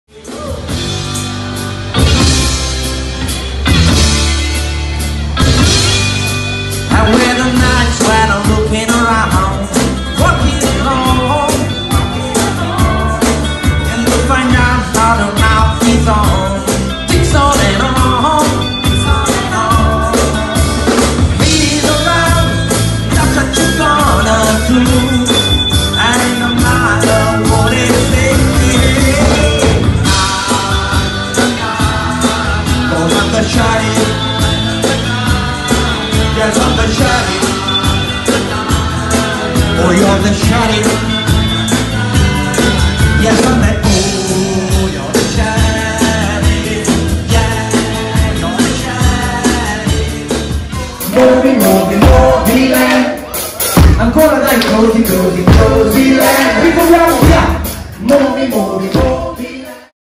Los Hermanos Live @Movieland American Beer Fest
Video promo American Beer Fest 2020 - Movieland Hollywood Park
Nostri ospiti sul palco i Rancheros!